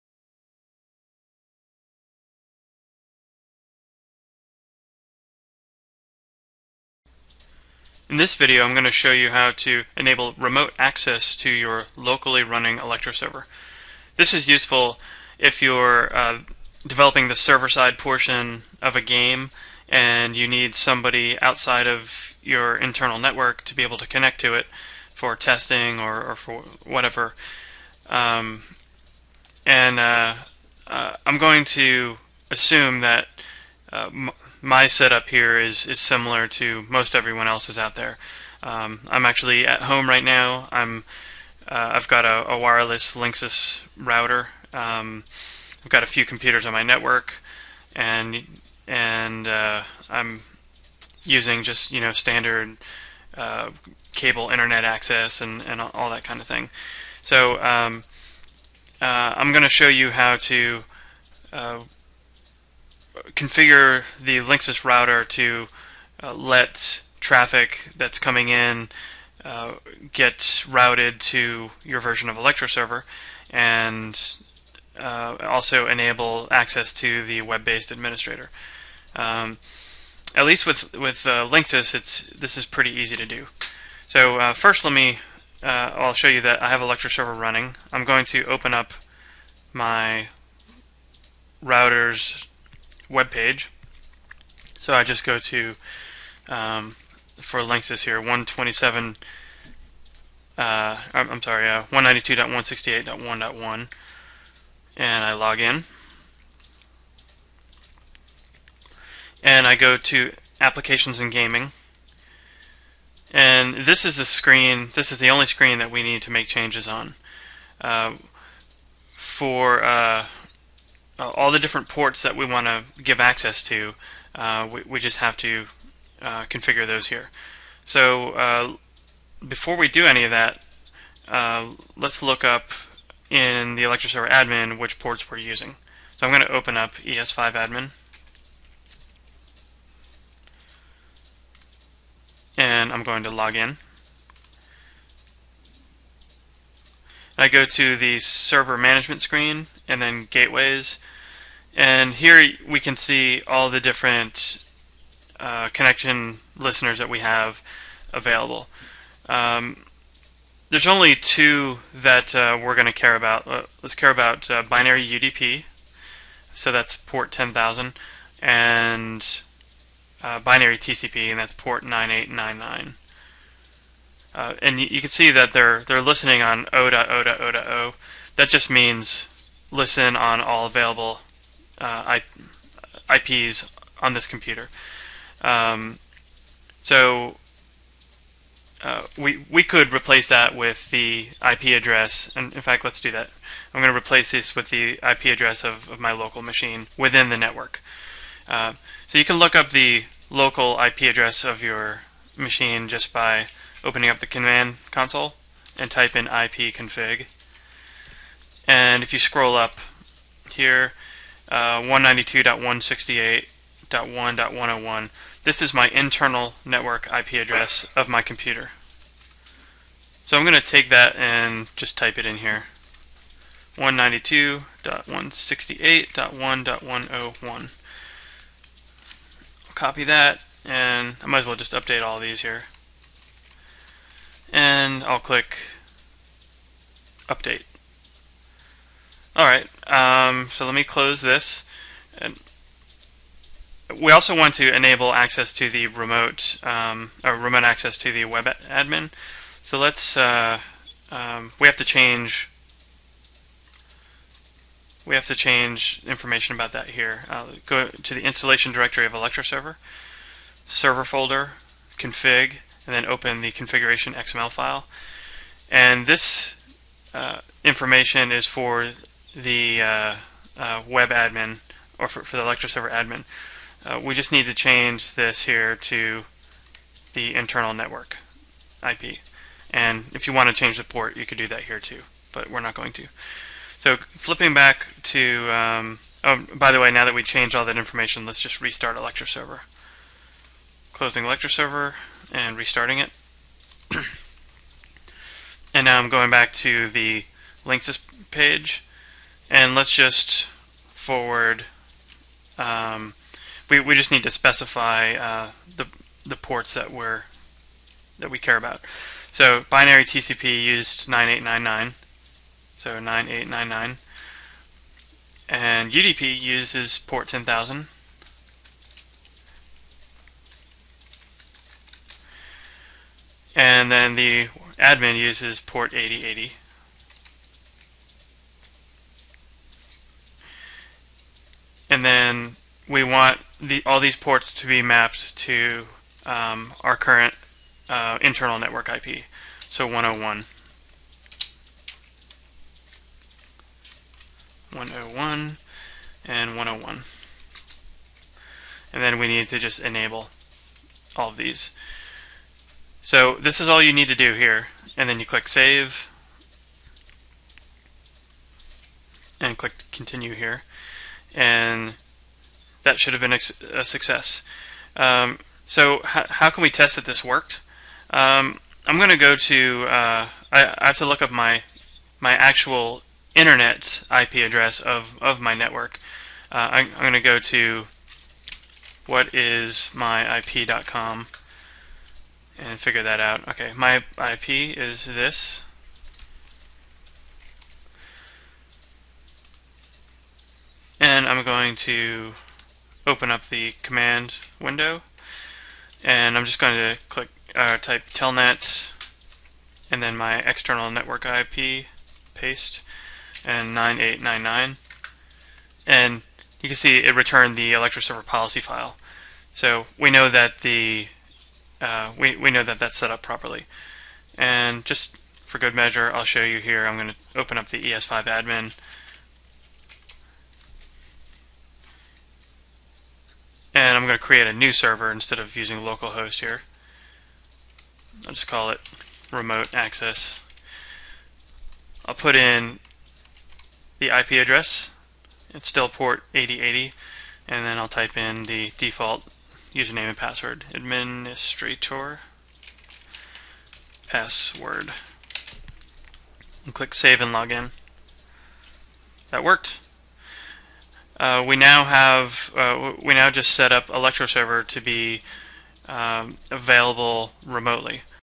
Enabling Remote Access to Local ElectroServer
This video shows you how to enable remote access to your locally running ElectroServer. This can be useful by allowing a developer to work on the server portion of a game locally while providing access for the client developer(s) to test against.